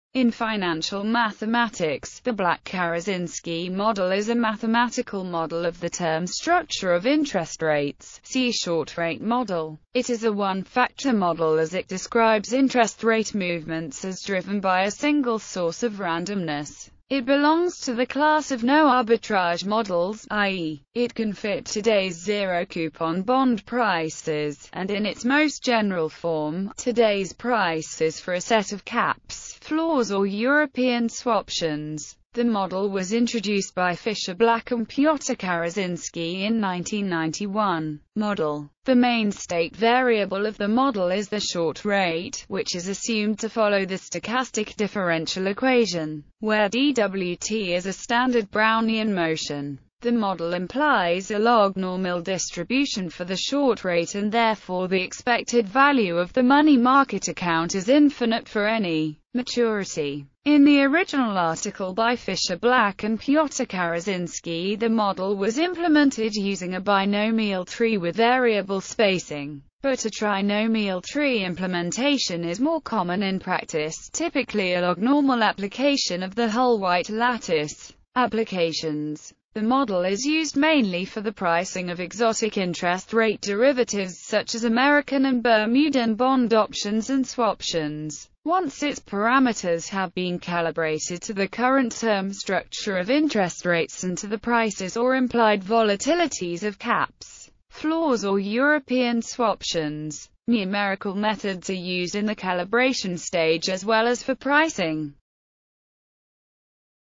Black–Karasinski model
In financial mathematics, the Black–Karasinski model is a mathematical model of the term structure of interest rates; see short rate model.It is a one-factor model as it describes interest rate movements as driven by a single source of randomness.